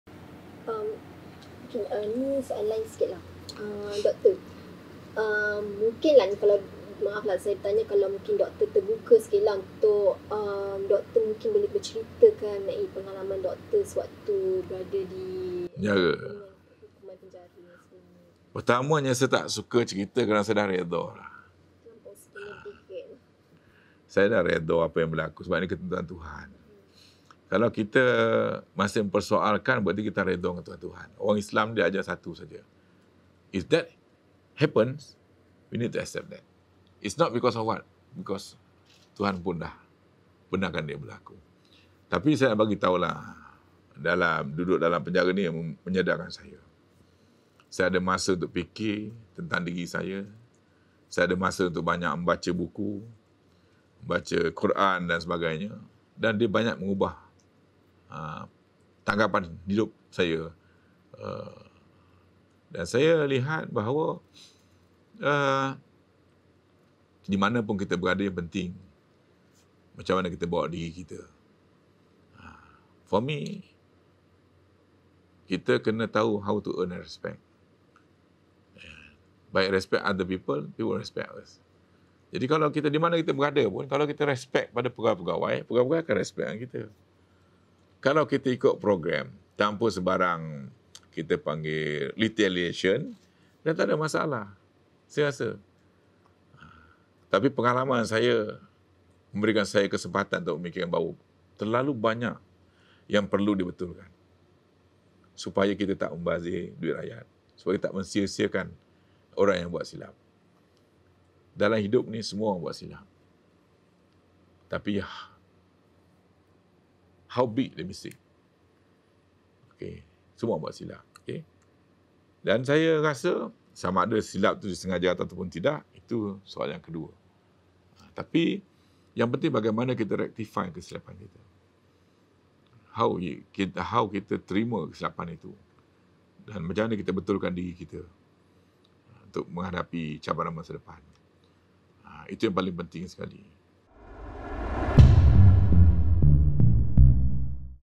Penjara mengajar saya erti tak bazirkan duit rakyat - Khir
Beberapa tahun lalu, bekas menteri besar Selangor Dr Mohamad Khir Toyo pernah menjalani hukuman penjara selepas disabit bersalah atas pertuduhan rasuah.

Mengimbas kembali perkara itu dalam wawancaranya bersama Malaysiakini, dia berkata penjara telah mengajarnya erti tidak membazirkan duit rakyat.